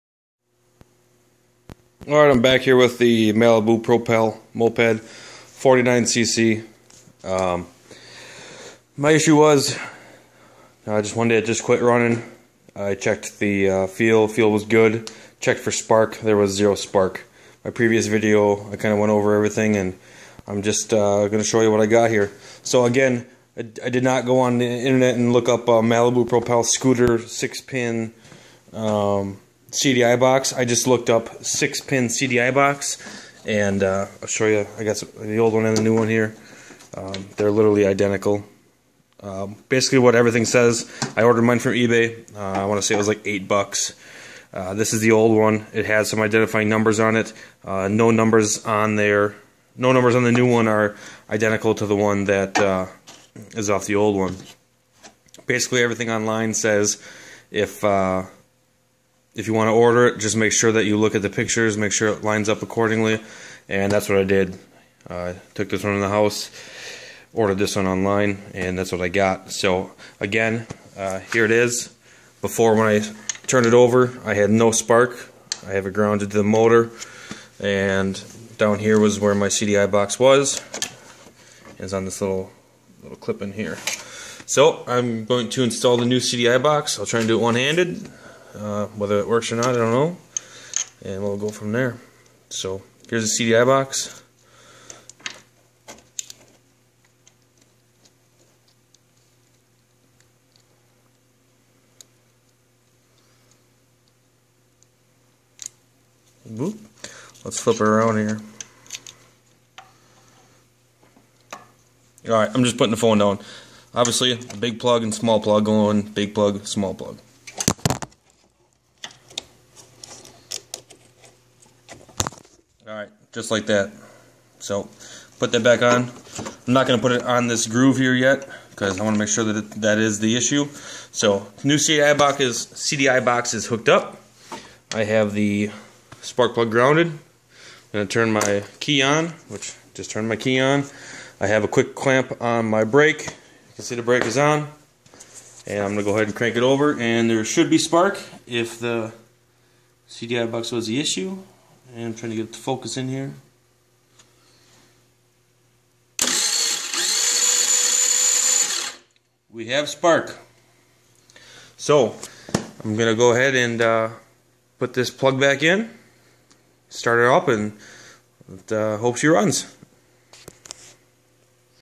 Chinese Scooter 49cc CDI install / replacement
Moped had no spark. Moped had fuel. Bought a new CDI box on ebay for less than $10 and installed it. The moped has spark and with a new plug started right up. Thanks Any questions ask.